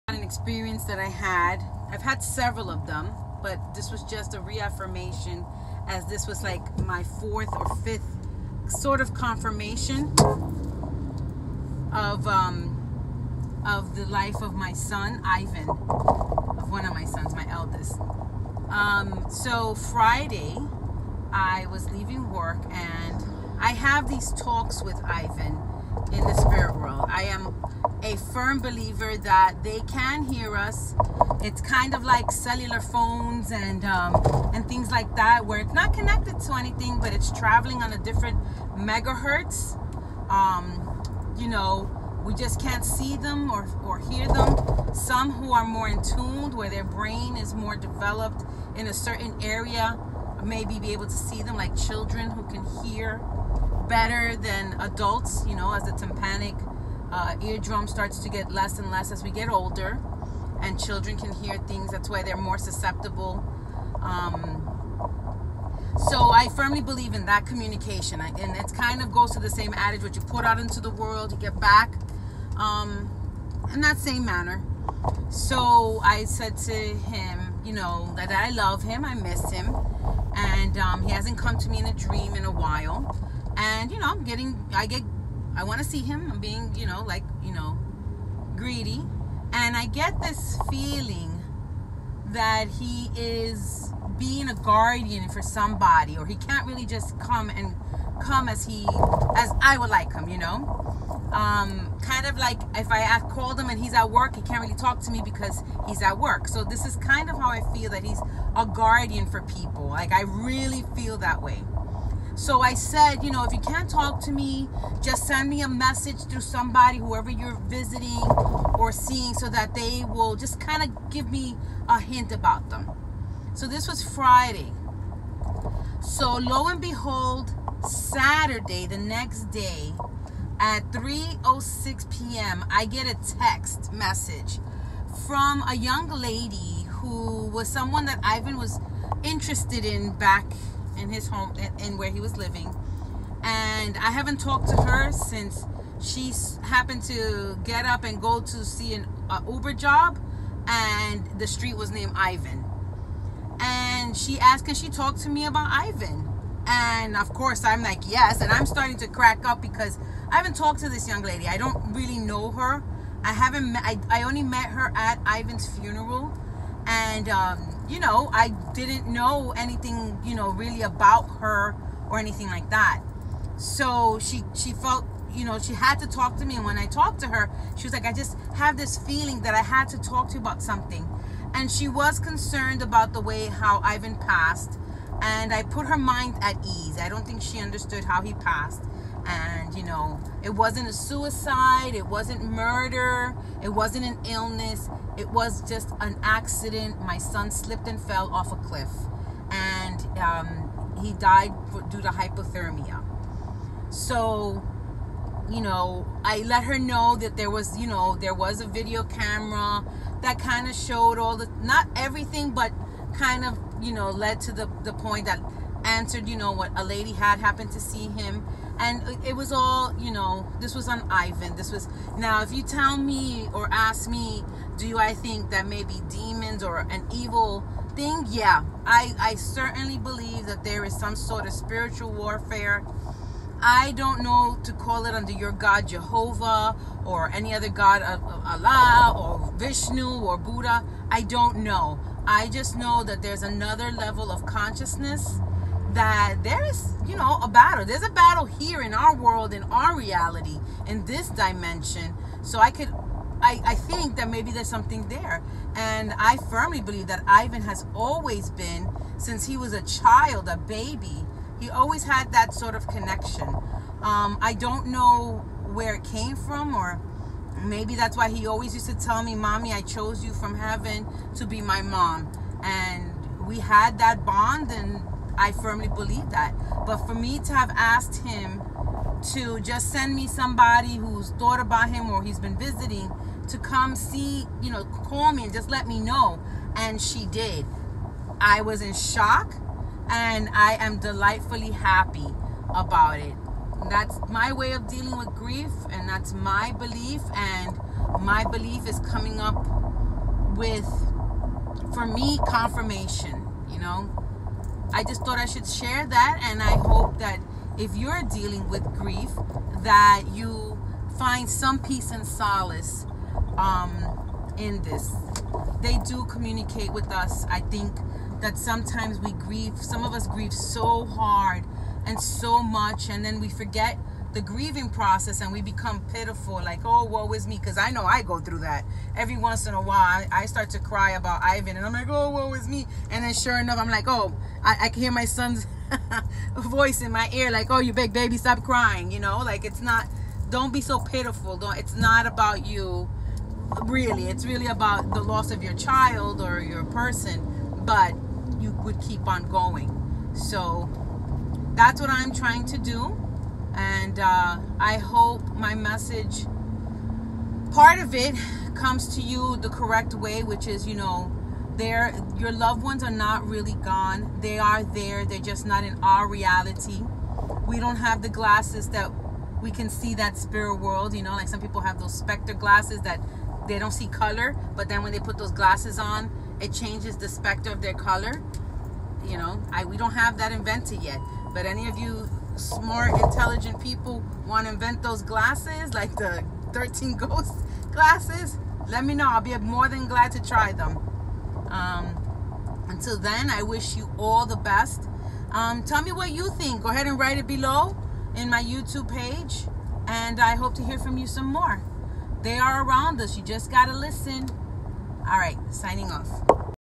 My son finding ways to communicate from the other side
I asked for a sign and I got what I was asking for